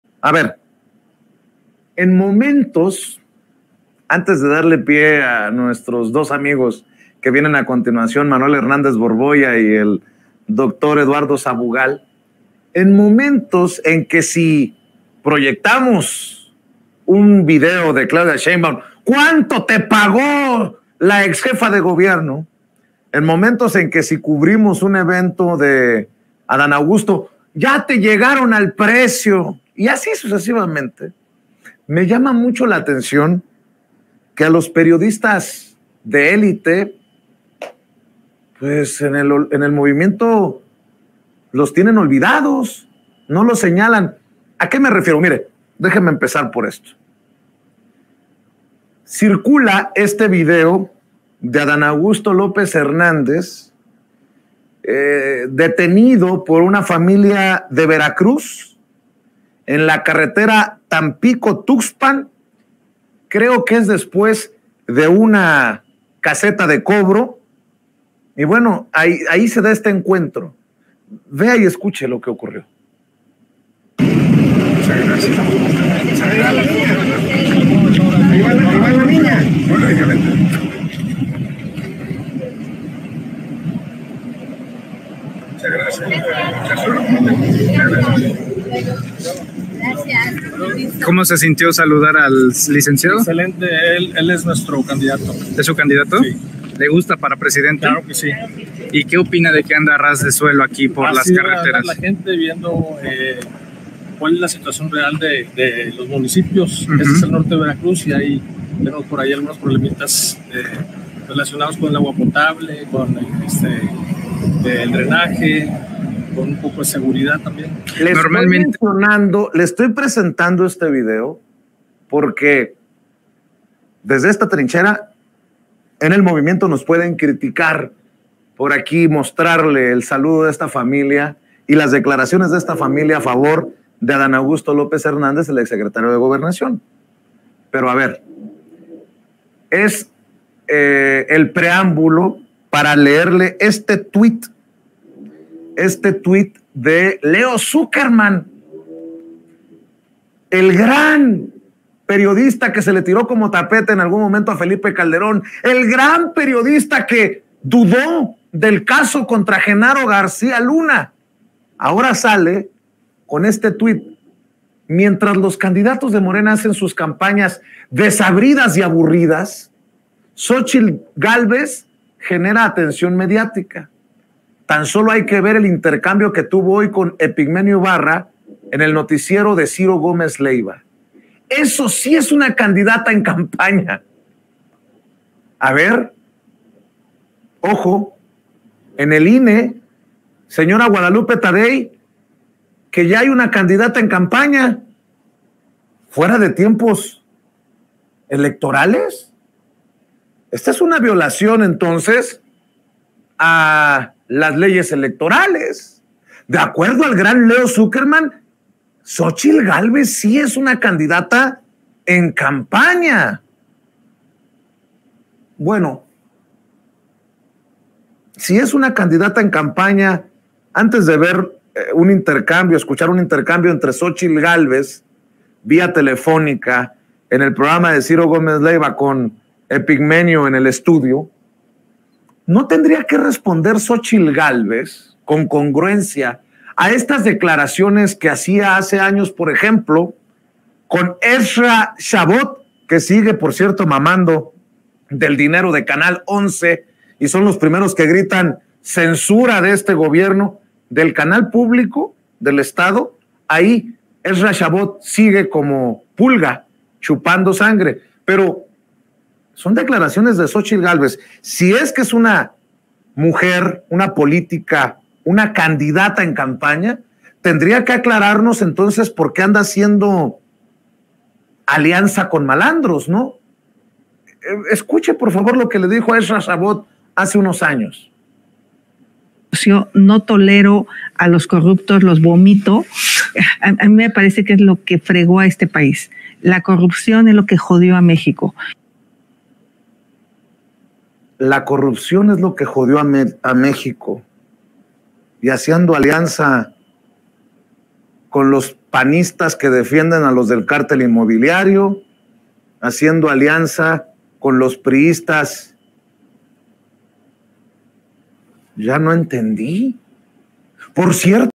¡ALERTA INE! DE ACUERDO A ZUCKERMANN, XÓCHITL YA ES UNA CANDIDATA EN CAMPAÑA
En momentos en que si proyectamos un video de Claudia Sheinbaum nos cuestionan que cuánto nos pago, o en el que si cubrimos un evento de Adán Augusto López nos acusan de que ya nos llegaron al precio, tendríamos que cuestionar el mieloso tweet que emitió Leo Zuckermann para ensalzar a Xóchitl Gálvez a quien puso ya como candidata.